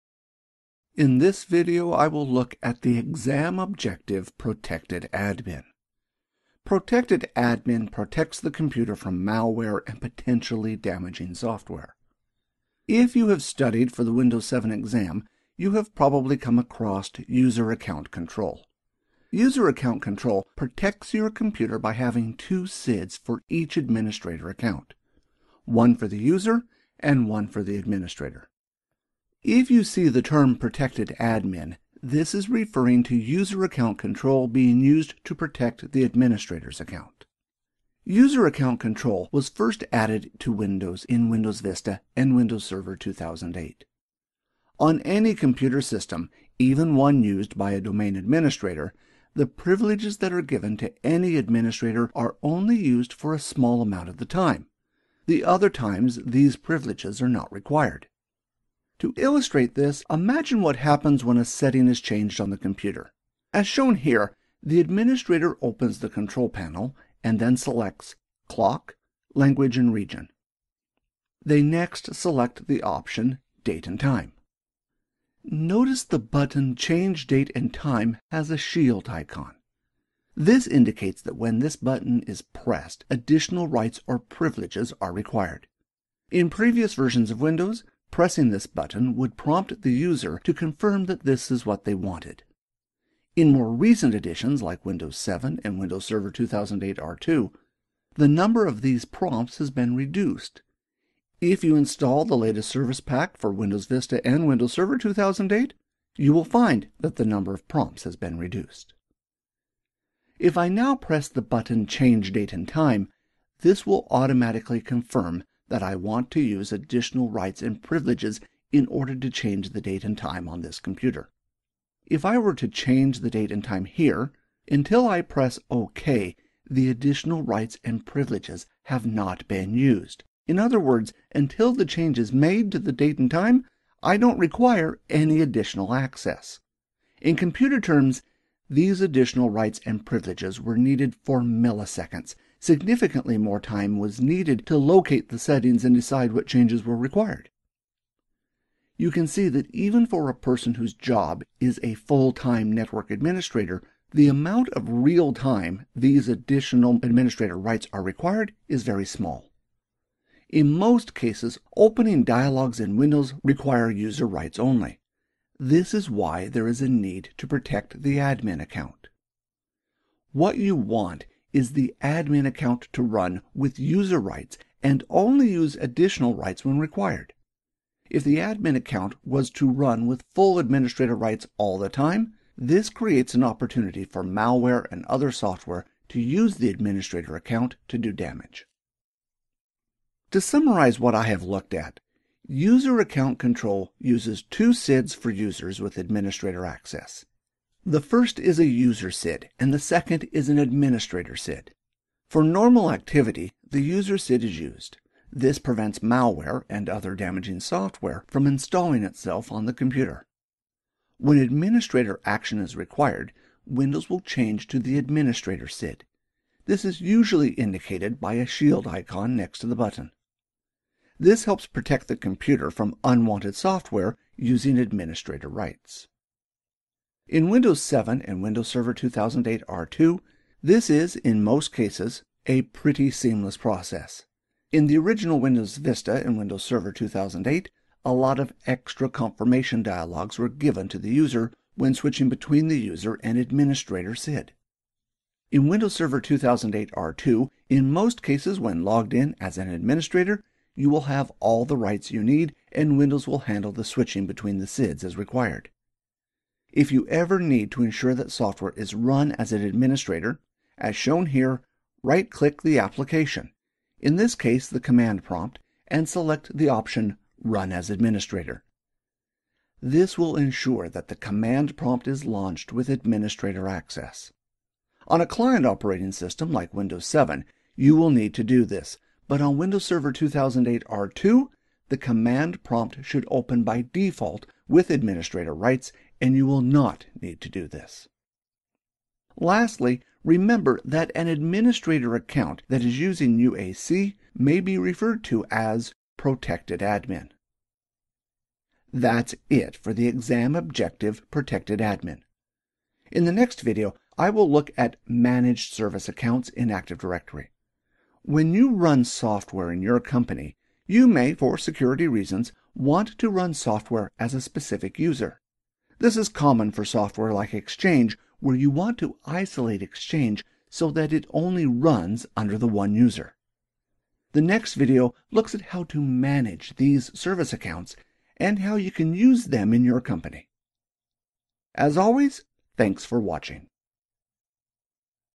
MCITP 70-640: Protected Admin
Protected Admin is essentially a term used to describe the administrator account being protected using User Account Control. This video looks at how User Account Control is used in Windows Server to protect the administrator's account.

User Account Control
User Account Control was first added in Windows Vista and Windows Server 2008. It addresses an issue that was common practice in Windows XP where a user would create an administrator account and use it for day to day activity. On most computer systems, the extra rights that an administrator account provides are only used for a small amount of time. For example, if you need to install software on the computer you would need administrator access to install the software, however to run the software it is unlikely that you would require administrator access. User Account Control essentially divides the administrator account into two,a user part and an administrator part. For normal activity the user part of the administrator account is used. When administrator access is required, the administrator part of the user is used.

To force an application to run with administrator rights, right click the application and select the option "run as administrator".

for our always free training videos. This is only one video from the many free courses available on YouTube.